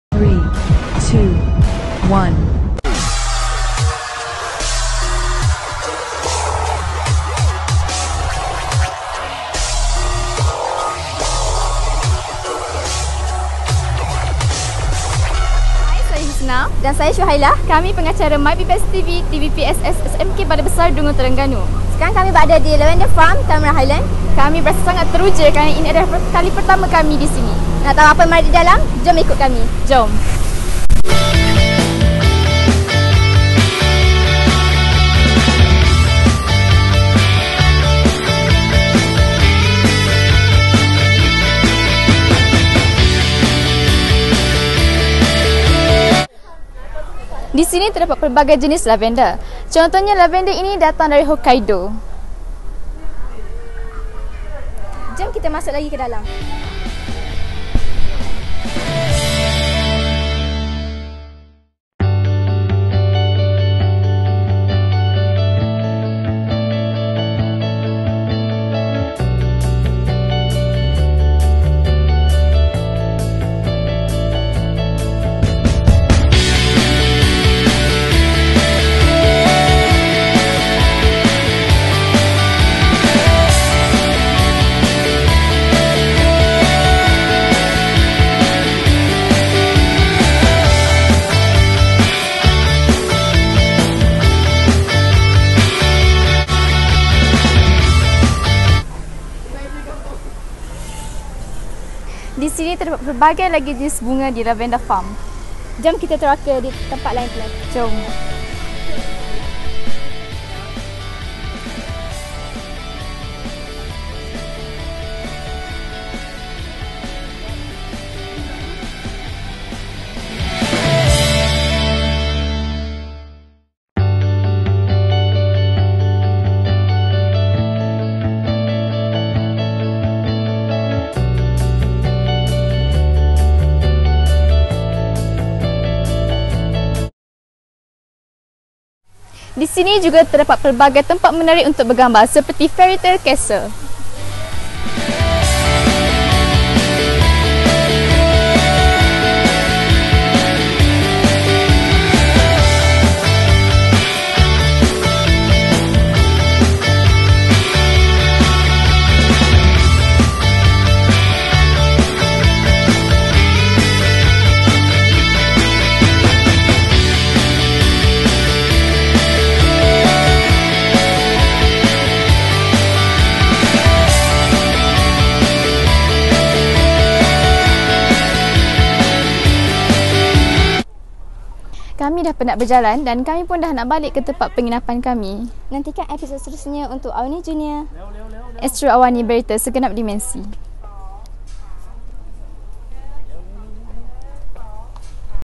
Episod 75: Husna dan Syuhailah di Lavender Farm Cameron Highland
Lavender farm dipenuhi dengan pokok-pokok lavender yang pelbagai jenis. juga terdapat pelbagai jenis bunga dan taman yang canik. jom tonton episod ini